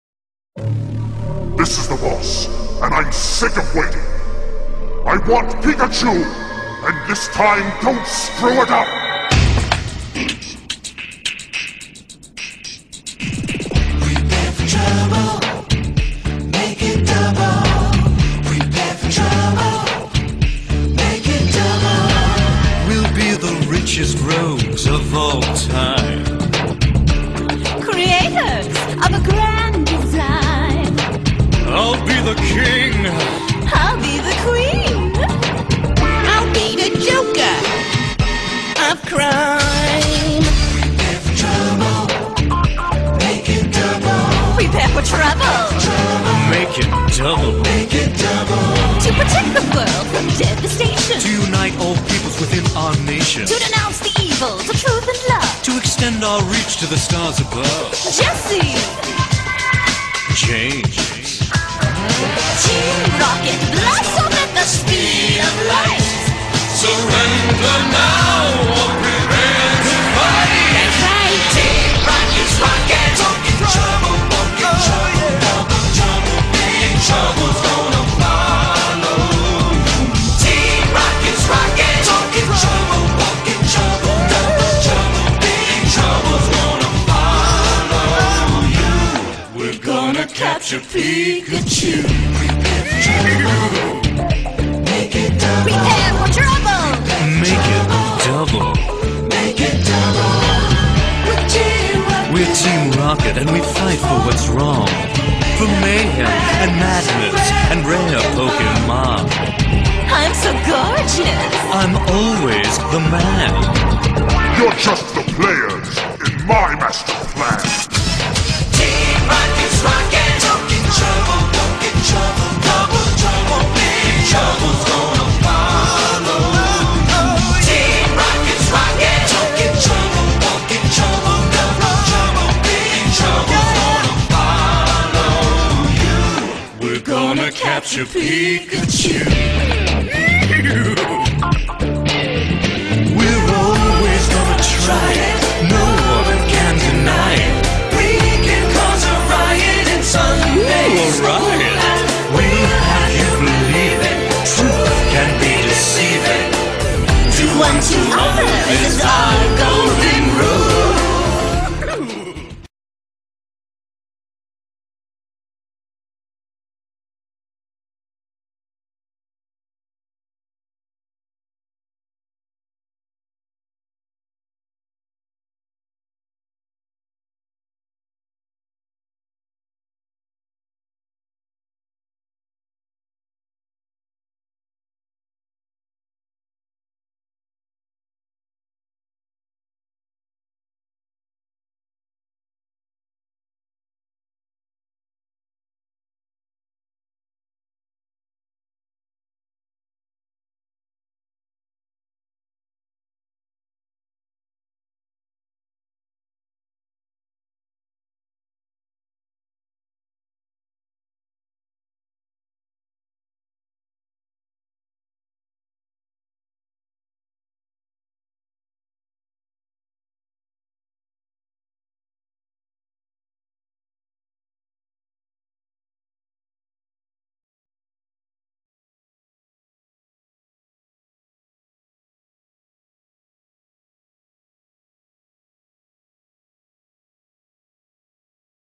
Team Rocket ~Speedpaint (Cat Form)
Thats how I image Jessie and James in Kitty form.